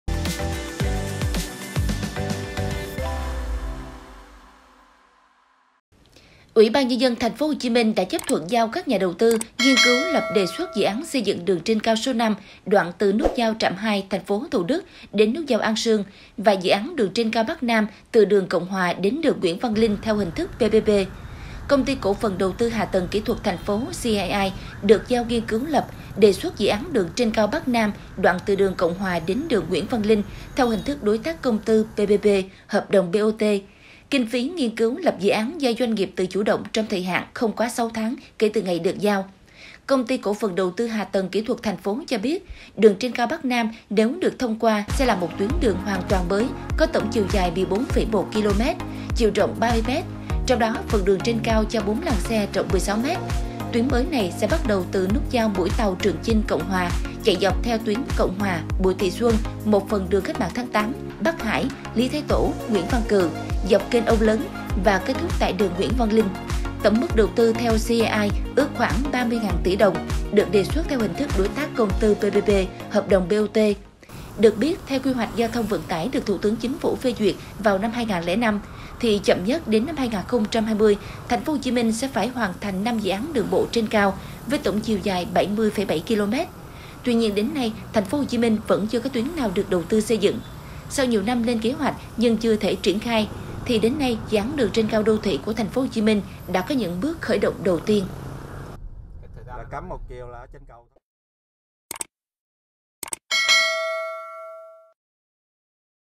TP.HCM: 30.000 TỶ ĐỒNG XÂY DỰNG ĐƯỜNG TRÊN CAO
UBND TP.HCM đã chấp thuận giao các nhà đầu tư nghiên cứu lập đề xuất dự án xây dựng đường trên cao số 5 đoạn từ nút giao Trạm 2 (TP Thủ Đức) đến nút giao An Sương và dự án đường trên cao Bắc - Nam từ đường Cộng Hòa đến đường Nguyễn Văn Linh theo hình thức PPP.